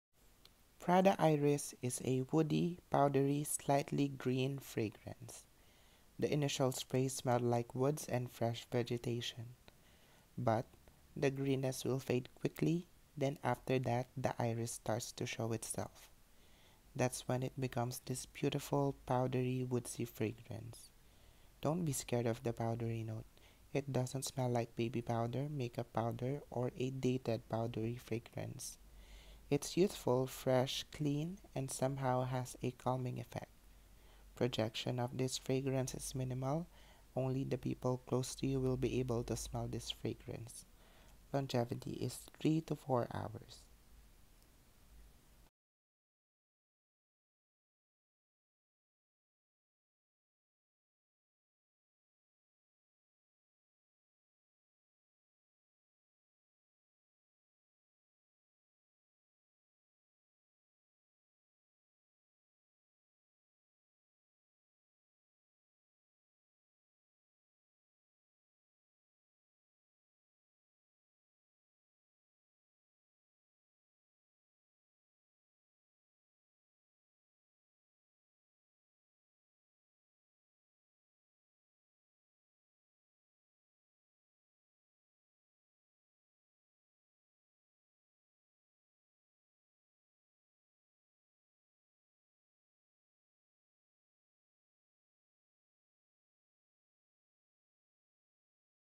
PRADA INFUSION D’IRIS FRAGRANCE REVIEW
This is my review of Prada Infusion D’Iris and showcase of the box and bottle for people who might be interested to see what an authentic looks like.(tester bottle)